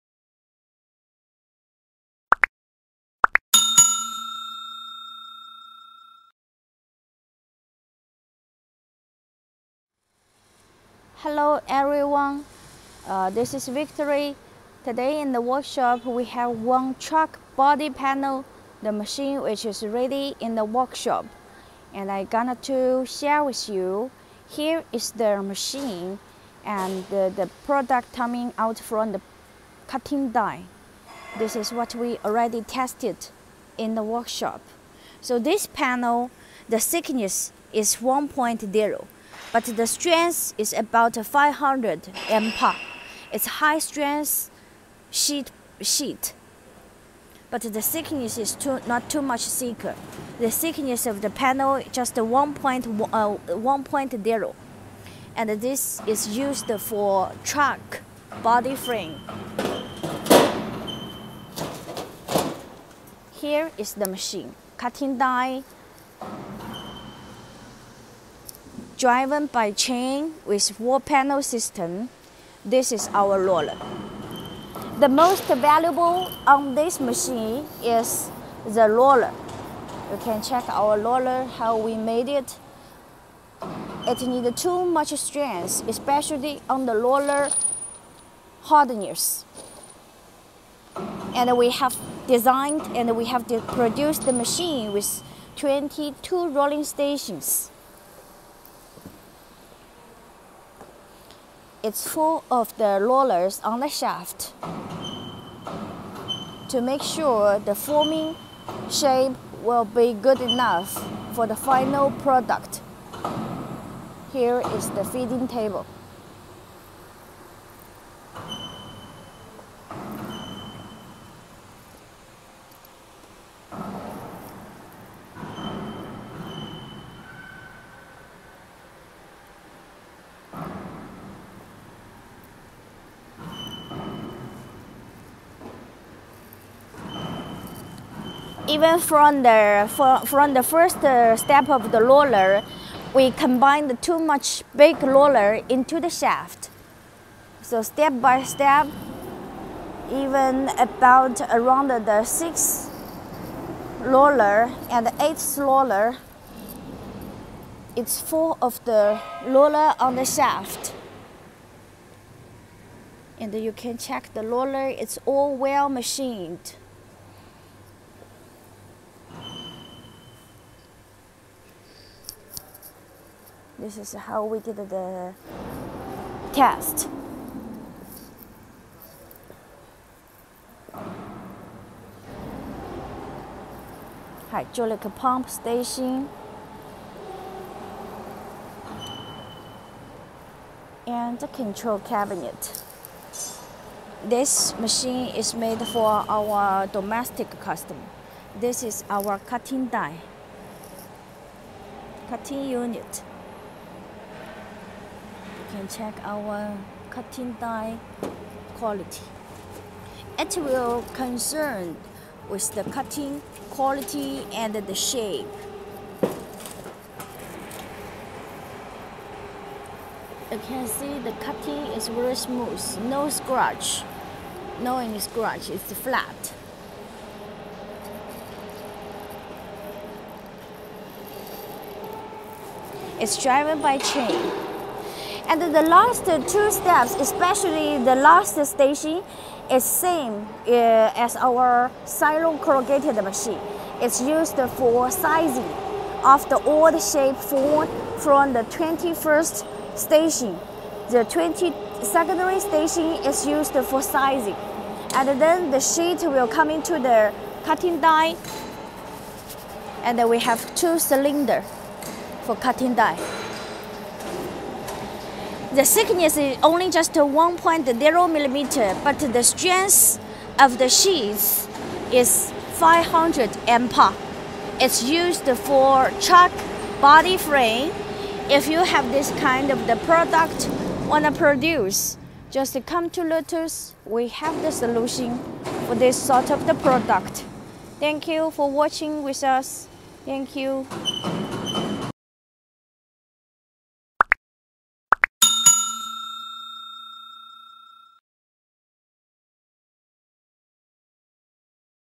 ▶ Body Car Panel Machine ◀ Container panel roll forming machine | Truck body panels machine
We can produce various kinds of Body Car Panel Machine and Car panel roll forming machine according to different usage requirements.
manufacture of roll forming machine